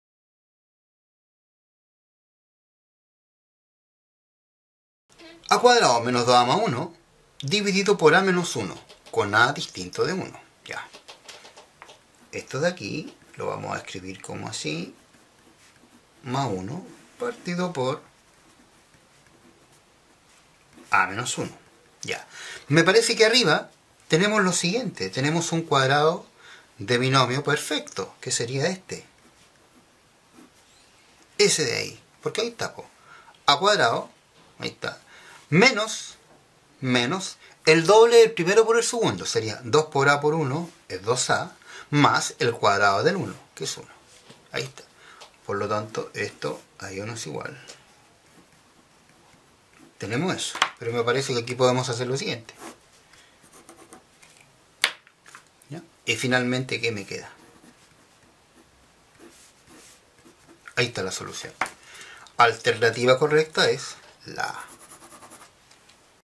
Álgebra y Funciones - Expresiones algebraicas - Problema 34
Álgebra y Funciones - Expresiones algebraicas

Problema 34

Para el contenido de expresiones algebraicas.

Una expresión algebraica contiene letras, números y signos. La manipulación de expresiones algebraicas tiene las mismas propiedades que la manipulación de expresiones numéricas, ya que las letras se comportan como si fuesen números. Las expresiones algebraicas que se tratarán en estos ejercicios, por lo general, una o dos letras.

Se contempla

Operatoria con términos semejantes
Factorización
Cuadrado del binomio
Suma por su diferencia
Productos notables
Anulación de una fracción
Indeterminación de una fracción